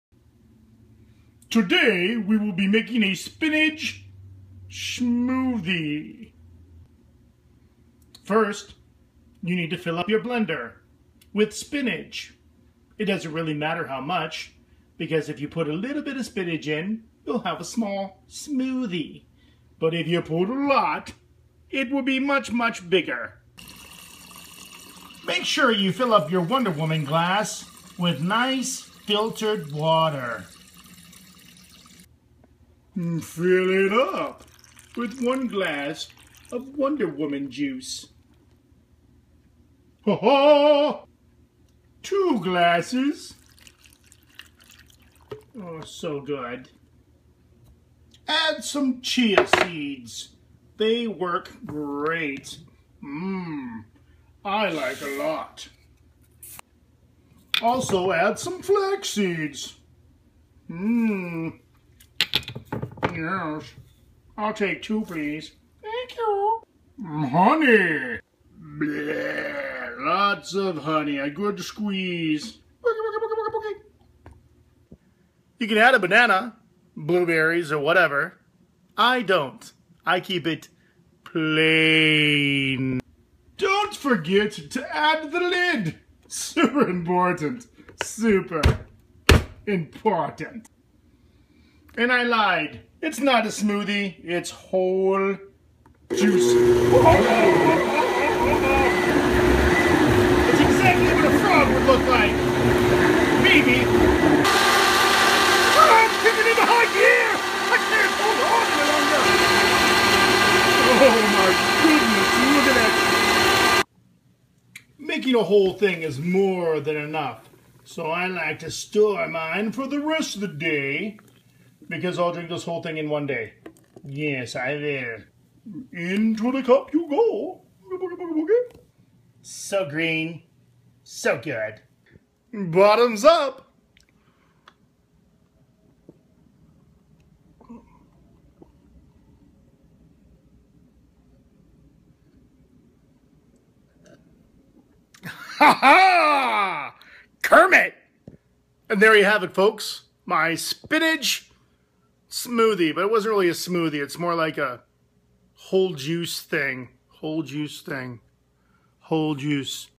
Cooking a dish with bliss ep02 Spinach smoothie/whole juice health drink
Eating healthy is good for you and can be a lot of fun. Especially if you use Wonder Woman juice.